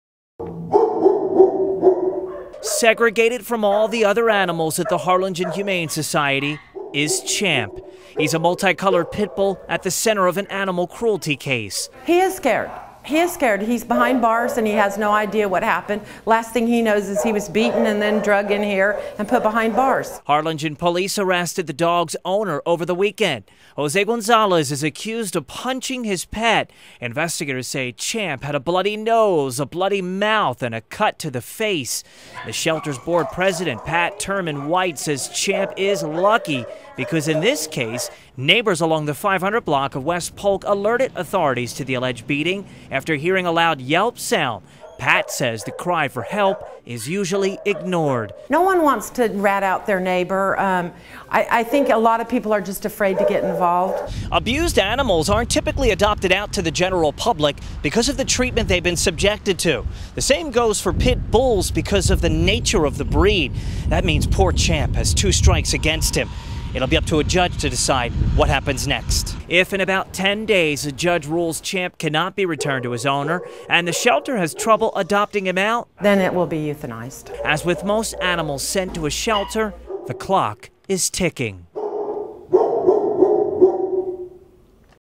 Humane society gives Champ a second chance
Segregated from all the other animals at the Harlingen Humane society is Champ. He's a multi-colored pitbull at the center of a cruelty to animals case.
     Harlingen police arrested the dog's owner over the weekend. Jose Gonzales is accused of punching his pet. Investigators say Champ had a bloody nose, a bloody mouth, and a cut to the face.
     The shelter's board president, Pat Turman-White, says Champ is lucky because in this case, neighbors along the 500 block of West Polk alerted authorities to the alleged beating after hearing a loud yelp sound. PAt says the cry for help is usually ignored.
     If in about 10 days a judge rules Champ cannot be returned to his owner, and the shelter has trouble adopting him out, then he will be euthanized.
     As with most animals sent to a shelter, the clock is ticking.